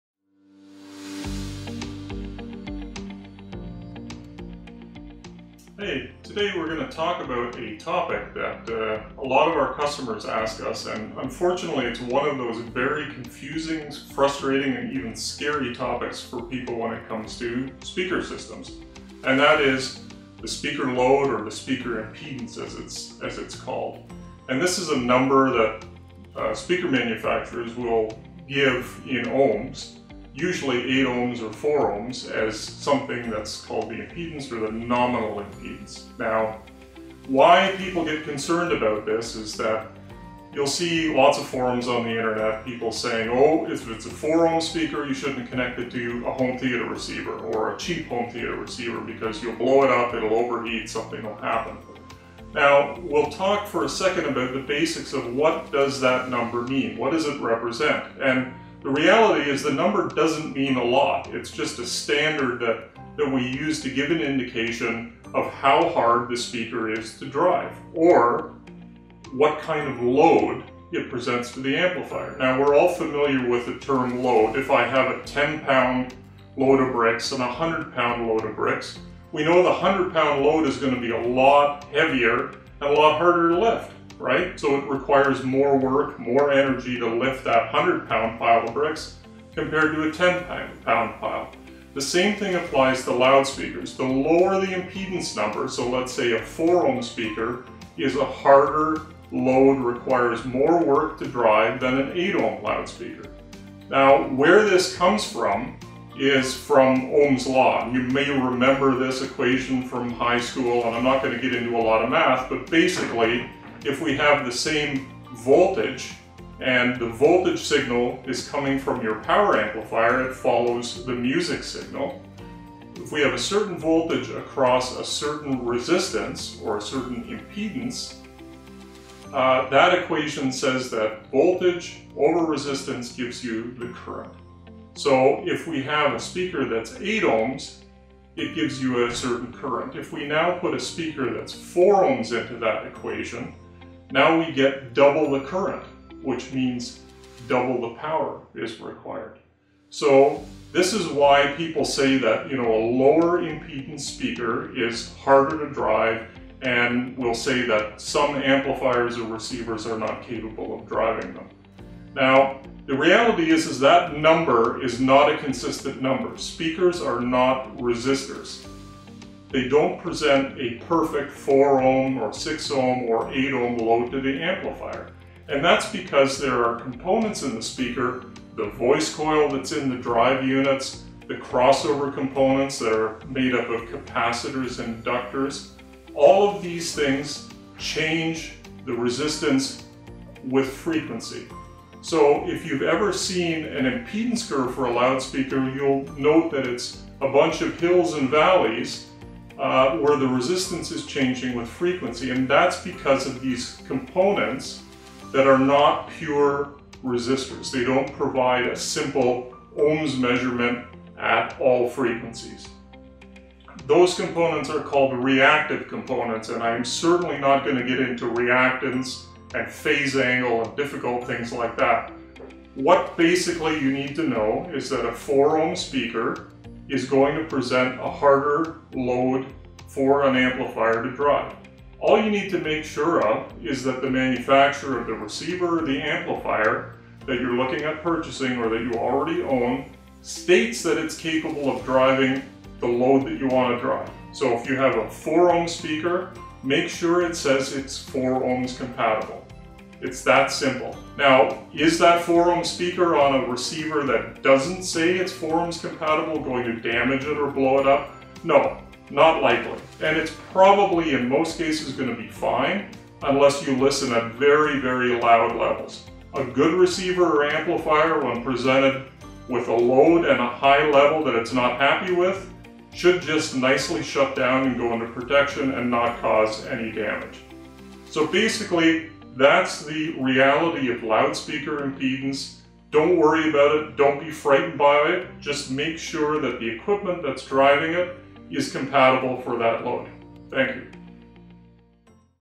Speaker Impedance Explained: Do You Need to Match Your Amplifier?
In this video, learn what speaker impedance means and how to prevent damaging speakers or amplifiers by mis-matching them.  Can you use 4ohm speakers with an amp rated at 8ohms?  Andrew Welker explains.

00:00 Speaker Impedance Explained
00:22 What does that number represent?
01:43 Ohm’s Law
02:41 Speakers are not resistors
03:42 Basic thing you need to know

Questions?